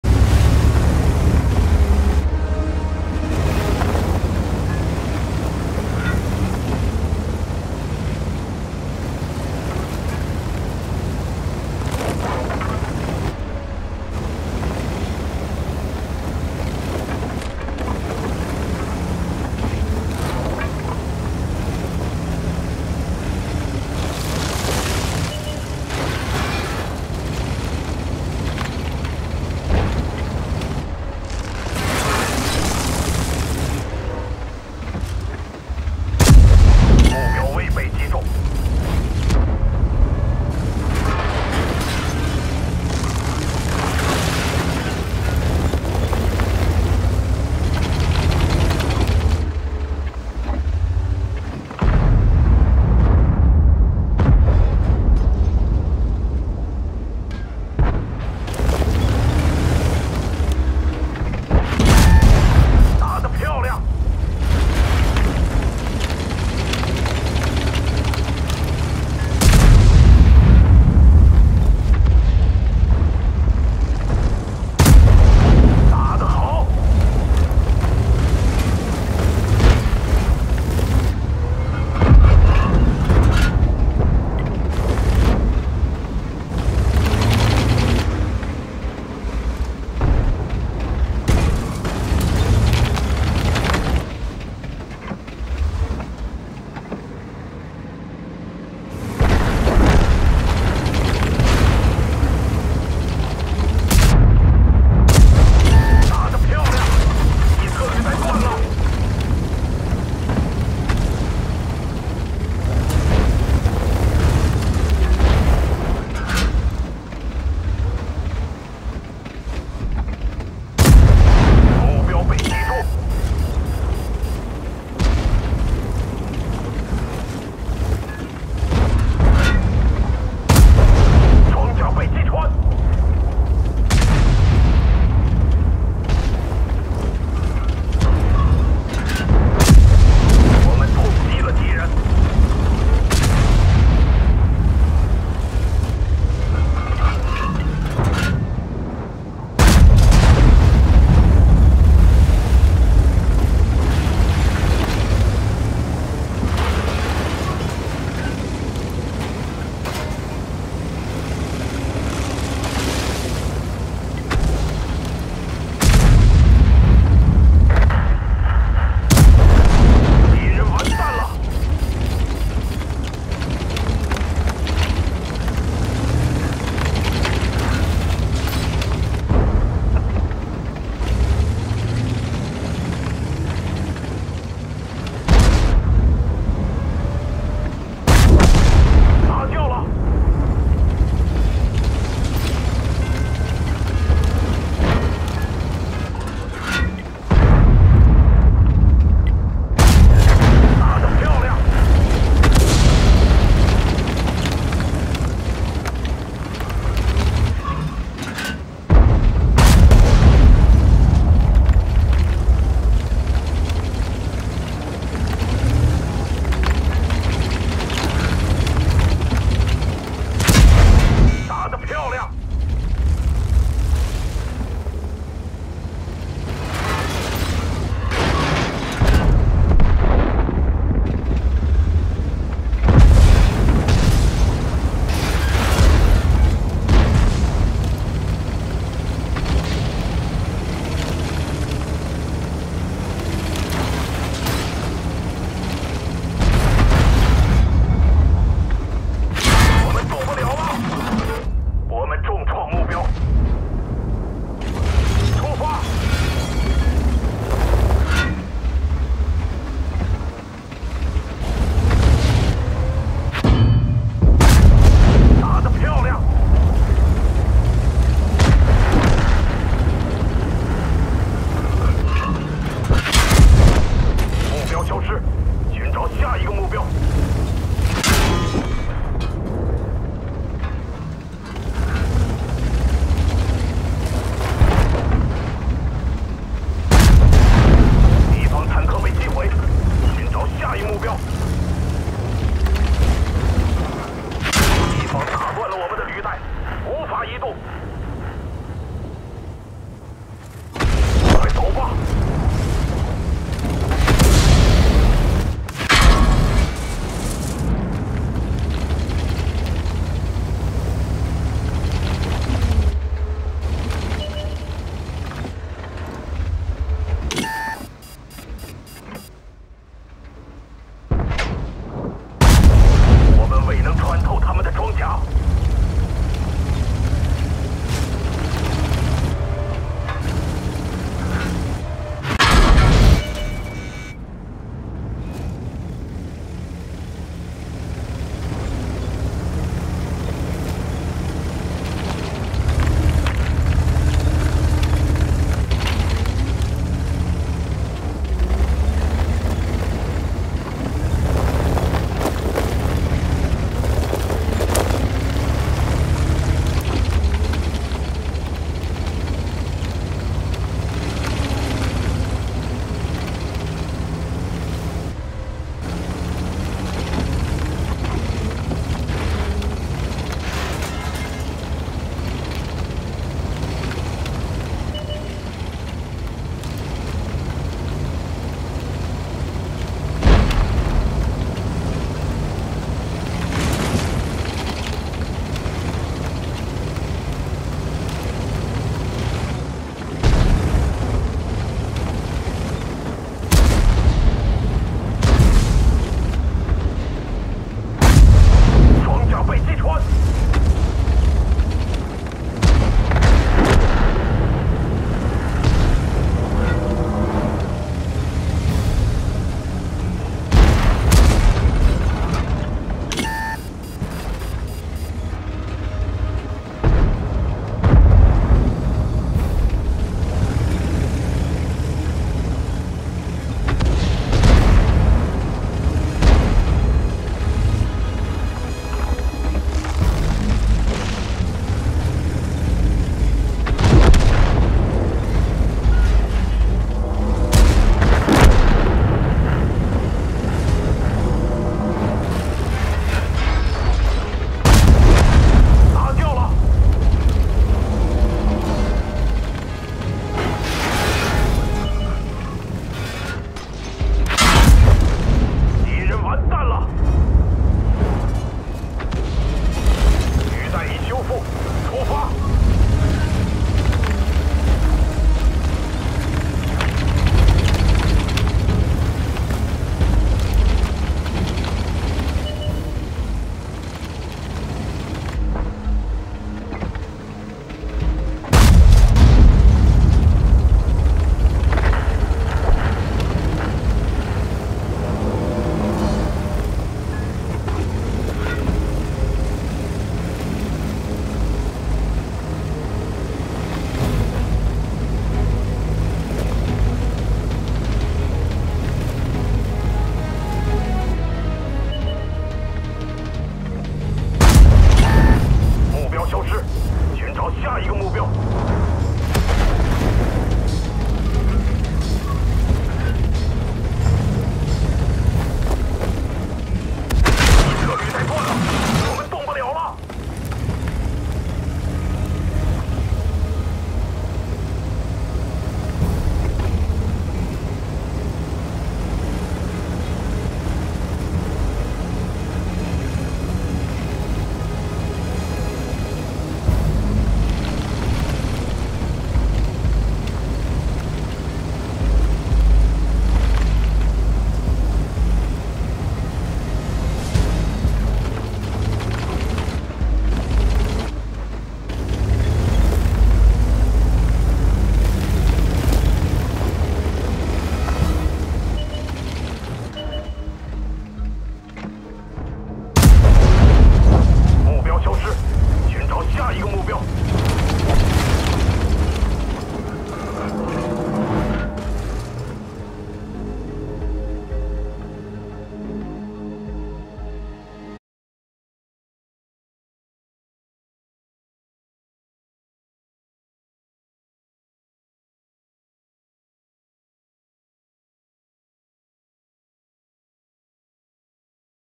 WZ-111 model 5A, Л. Зигфрида, Стандартный бой
Игрок: TwoStepsFromHelI